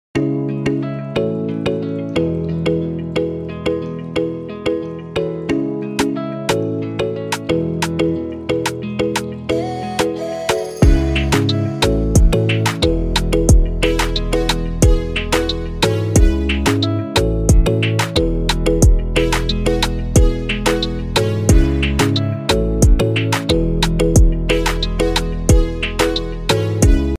IKEA Finds😍
This IKEA Decorative Lights collection will instantly bright up any room.